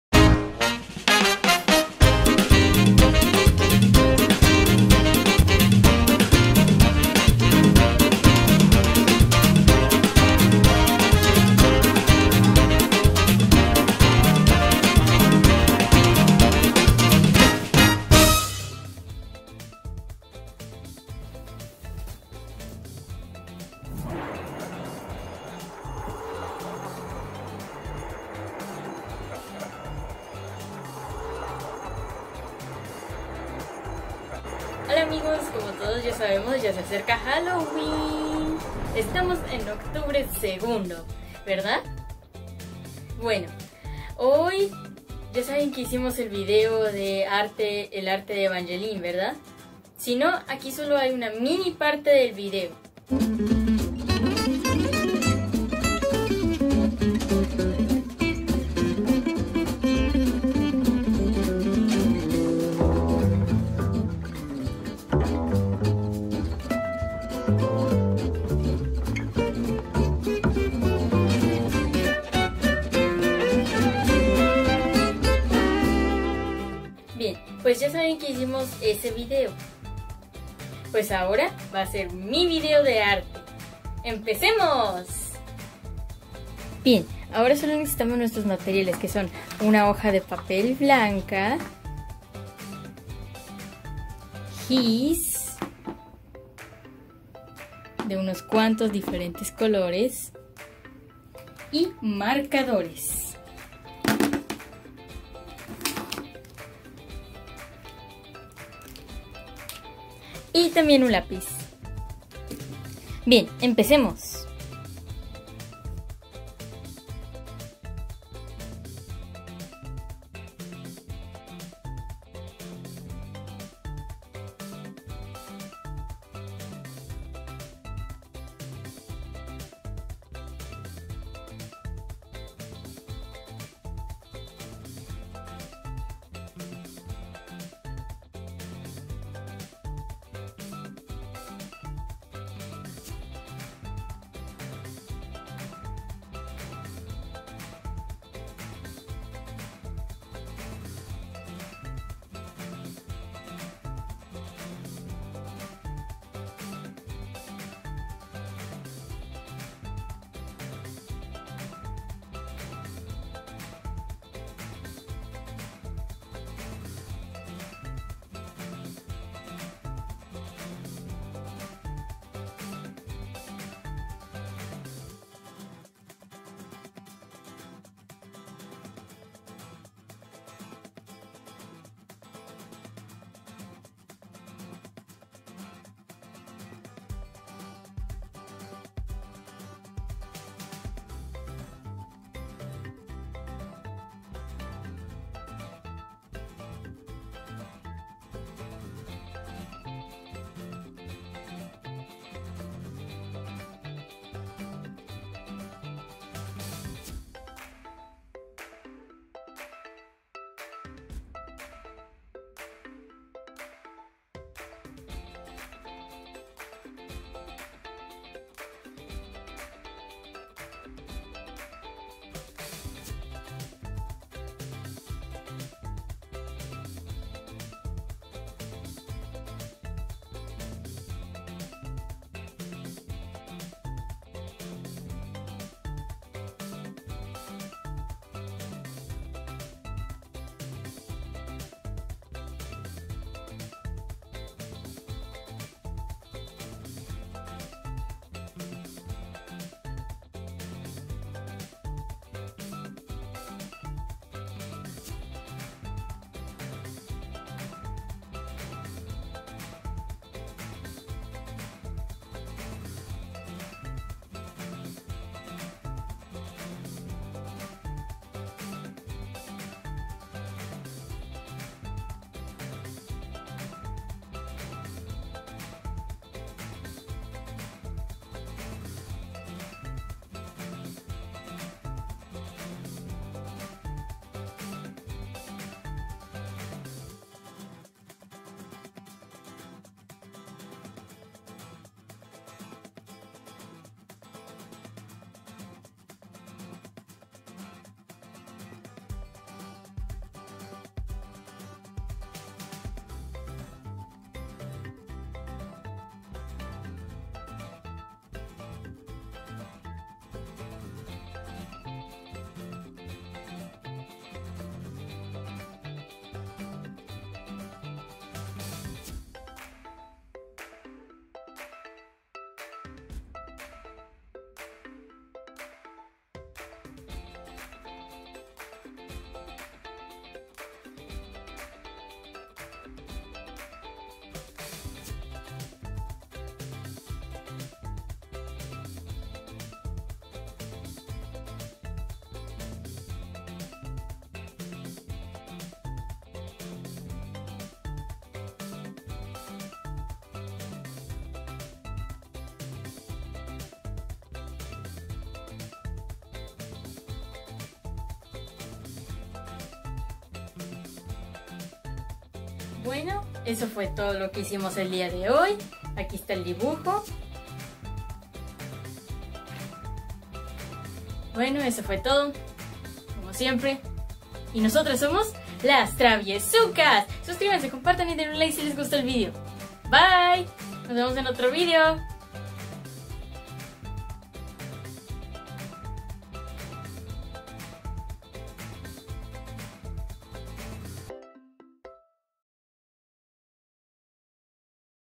El Arte de Valeria !
Esta ves Valeria a hecho un dibujo 🎨🖌 y lo quiere compartir con ustedes ! Esperamos que les guste y que los disfruten ! Aqui esta el link de el video de Evangeline !